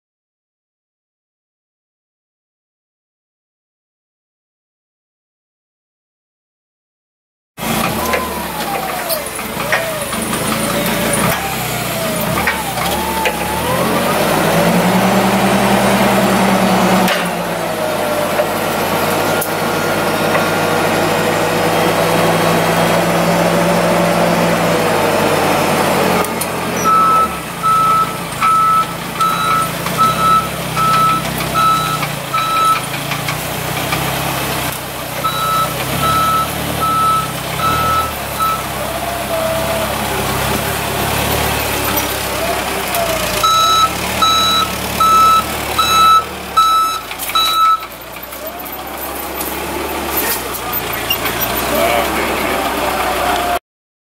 2009 Yale GLC040SVX forklift for sale | no-reserve Internet auction February 21, 2018
2009 Yale GLC040SVX forklift
1,608 hours on meter
Mitsubishi four cylinder propane engine
Powershift transmission
1F-1R gears
3,200 lbs max lift capacity
187" max lift height
Side shift
Mast tilt
42" fork length
18x7x12 1/8 front tires
15x5x11 rear tires
Propane bottle not included
Serial C809V04724G
It's located in Tonganoxie, KS, and we're selling it to the highest bidder regardless of price.